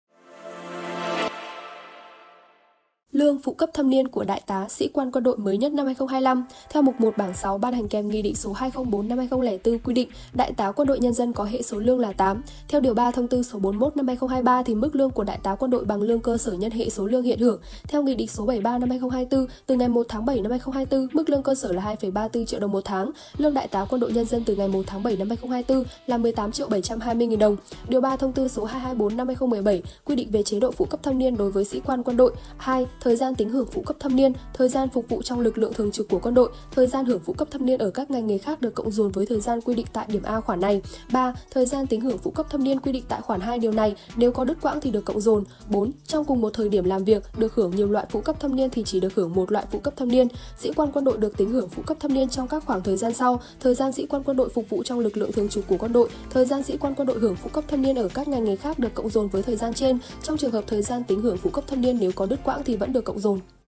Lương đại tá, phụ cấp thâm niên sĩ quan quân đội năm 2025| Báo Lao Động
BÁO LAO ĐỘNG |Lương Đại tá Quân đội nhân dân từ 1.7.2024 là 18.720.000 đồng. Chế độ phụ cấp thâm niên với sĩ quan quân đội quy định tại Điều 3 Thông tư 224/2017/TT-BQP.

* Đường dây nóng: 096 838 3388